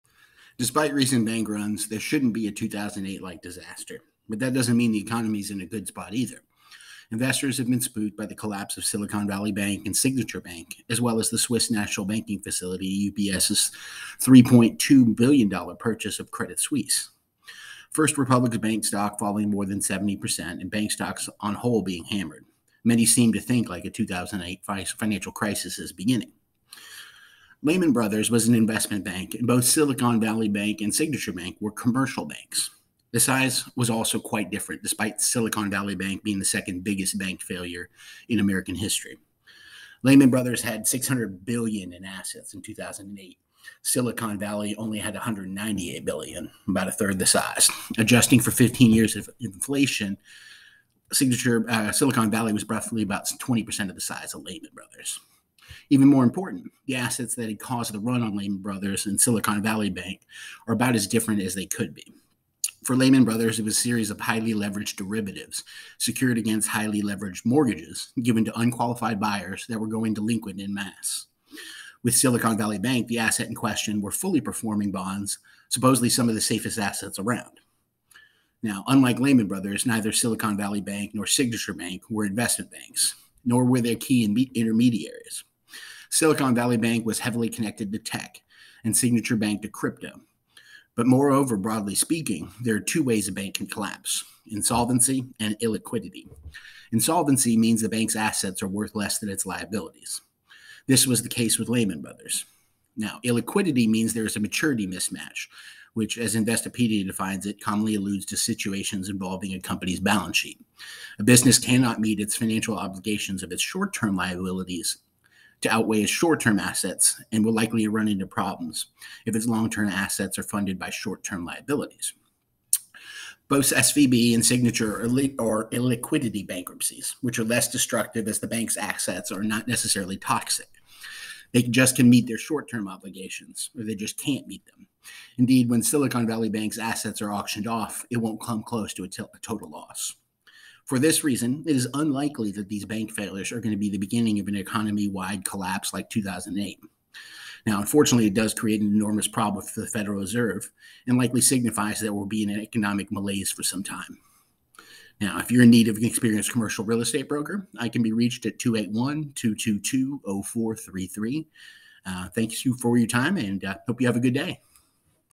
Are Banking Failures Going To Cause Another 2008 Like Crash?
Despite recent bank runs, there shouldn't be a 2008-like disaster, but that doesn't mean the economy is in a good spot. Investors have been spooked by the collapse of Silicon Valley Bank and Signature Bank, as well as the Swiss National Bank facilitating UBS’ $3.2 billion purchase of Credit Suisse, First Republic Bank’s stock falling more than 70%, and bank stocks, on the whole, being hammered. Many seem to think a 2008-like financial crisis is beginning.

Lehman Brothers was an investment bank, and both Silicon Valley Bank and Signature Bank were commercial banks. The size was also quite different despite Silicon Valley Bank being the second biggest bank failure in American history. Lehman Brothers had $600 billion in assets in 2008. Silicon Valley Bank had $198 billion. Adjusting for 15 years of inflation, Silicon Valley Bank was maybe 20% the size of Lehman Brothers.

Even more important, the assets that caused the runs on Lehman Brothers and Silicon Valley Bank are about as different as could be. For Lehman Brothers, it was a series of highly leveraged derivatives secured against highly leveraged mortgages given to unqualified buyers that were going delinquent en masse. With Silicon Valley Bank, the asset in question were fully performing bonds, supposedly some of the safest assets around.

Unlike Lehman Brothers, neither Silicon Valley Bank nor Signature Bank were investment banks, nor were they key intermediaries. Silicon Valley Bank was heavily connected to tech, and Signature Bank to crypto. But moreover, broadly speaking, there are two ways a bank can collapse: insolvency and illiquidity. Insolvency means the bank’s assets are worth less than its liabilities. This was the case with Lehman Brothers. Illiquidity means there is a “maturity mismatch,” which, as Investopedia defines it, “…commonly alludes to situations involving a company’s balance sheet. A business cannot meet its financial obligations if its short-term liabilities outweigh its short-term assets and will likely run into problems, too, if its long-term assets are funded by short-term liabilities.”

Both SVB and Signature are illiquidity bankruptcies, which are less destructive as the bank’s assets aren’t entirely toxic. They just can’t meet their short-term obligations. Indeed, when Silicon Valley Bank’s assets are auctioned off, it won’t come close to a total loss.

For this reason, it is unlikely these bank failures are the beginning of an economy-wide collapse like 2008. Unfortunately, it does create an enormous problem for the Federal Reserve and likely signifies we will be in an economic malaise for some time.

Commercial Real Estate Development eXp Commercial – Viking Enterprise Team Agent Attraction